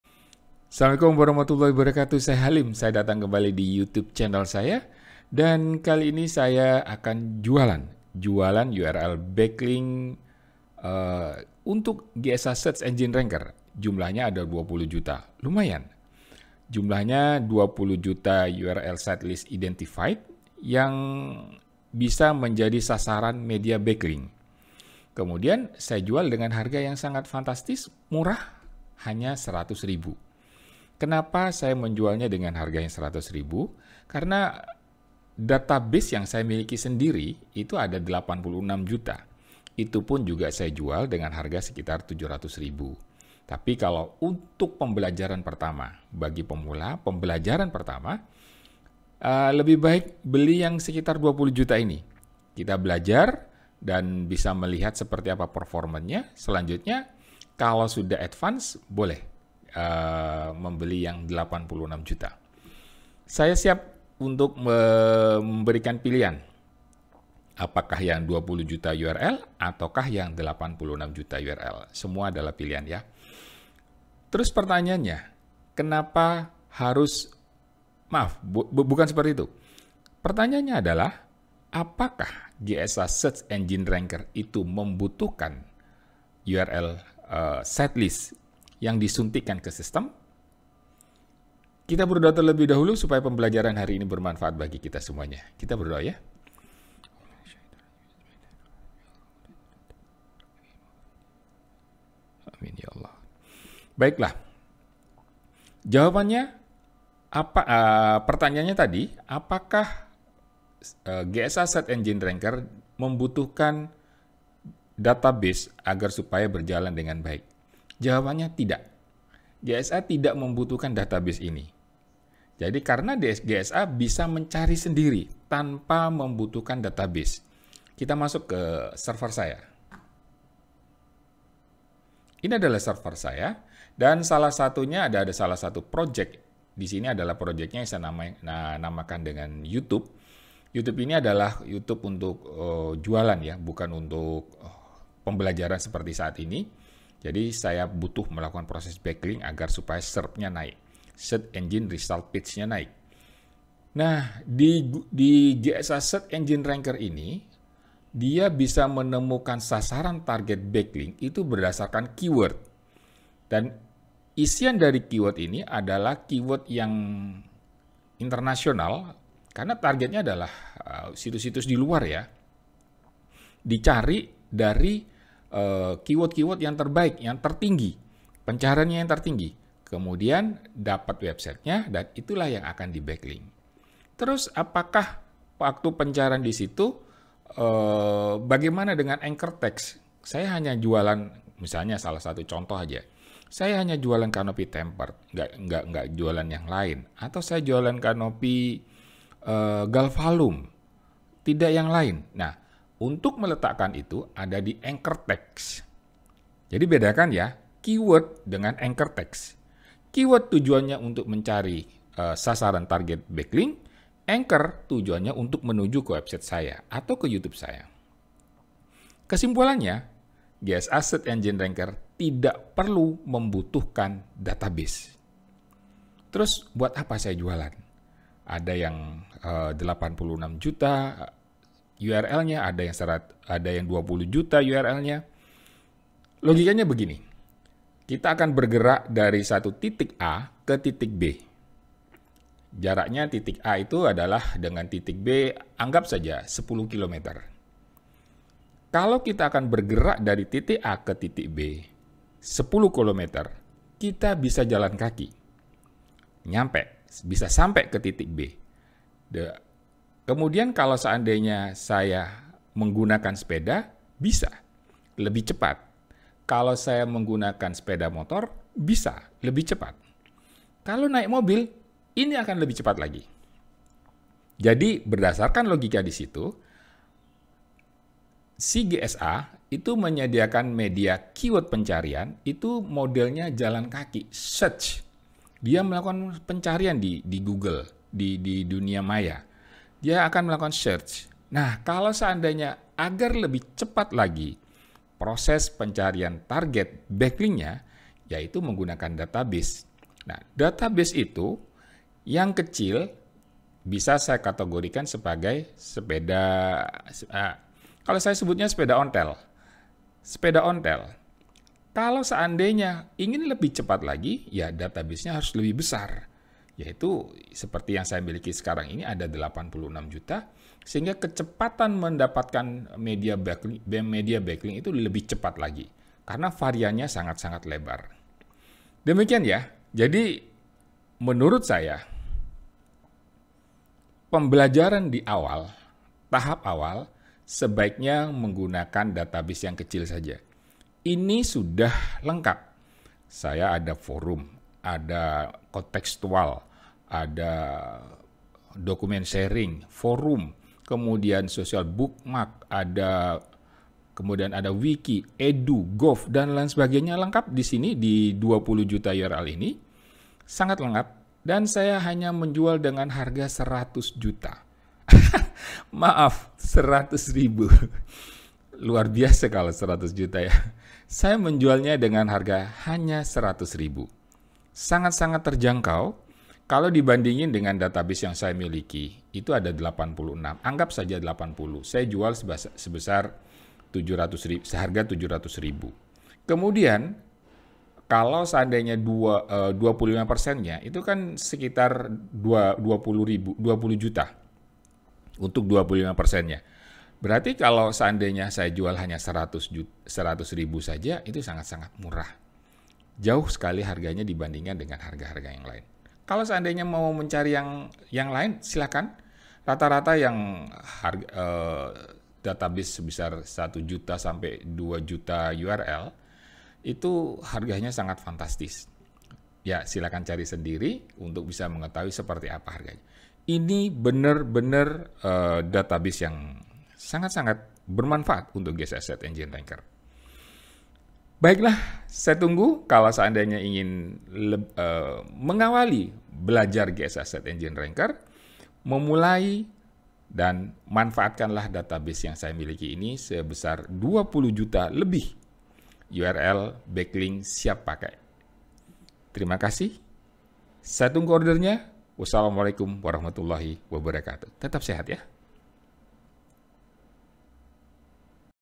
Backlink Berkualitas Murah | 20 Juta URL | GSA Search Engine Ranker
Backlink berkualitas dan murah pastilah menjadi idaman kita semua para Internet marketer.
Saya memiliki 20 Juta URL Backlink berkualitas siap pakai yang saya scrape dari database yang saya miliki, sebanyak 86 Juta URL.

Database 20Juta URL yang saya jual dengan harga hanya Rp 100.000 ini sudah lengkap, terdiri atas backlink contextual, WIKI, EDU, GOV dan lainnya, sehingga dengan menggunakan database ini proses backlink akan lebih cepat.